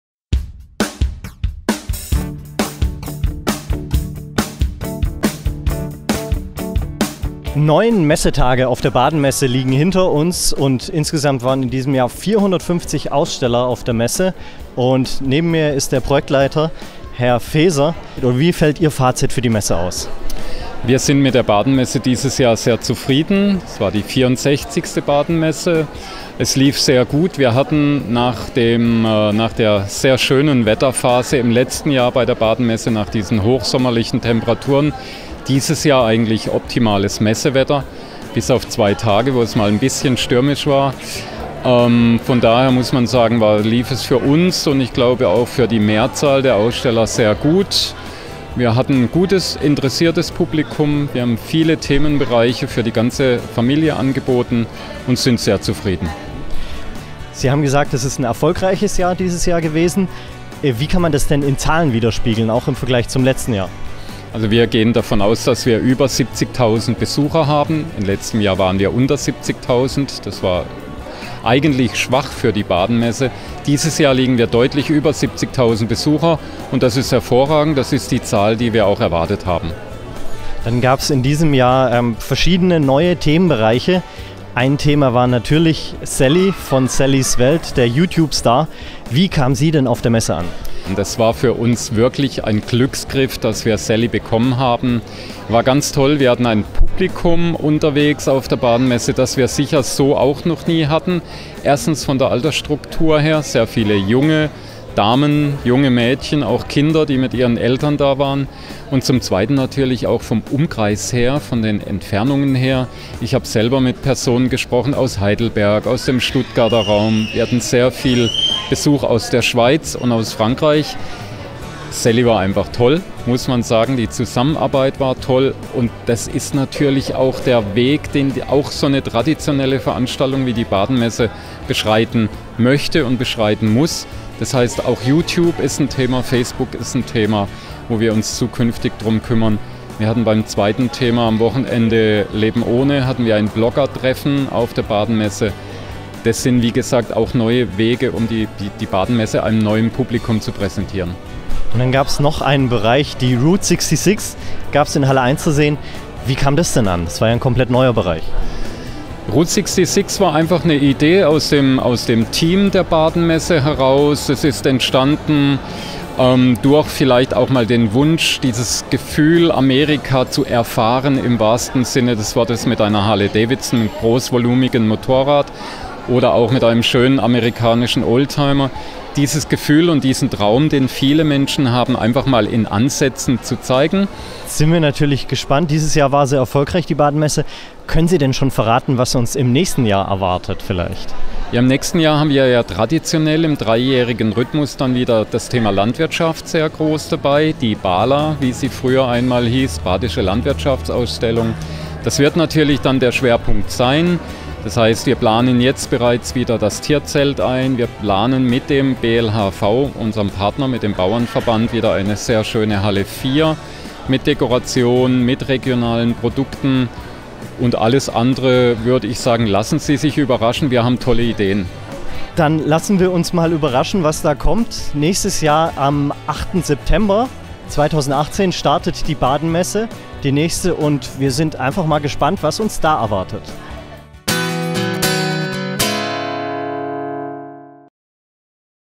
Das Fazit zur Baden Messe 2017!
Projektleiter Thomas Veser zieht ein Fazit zur Baden Messen 2017.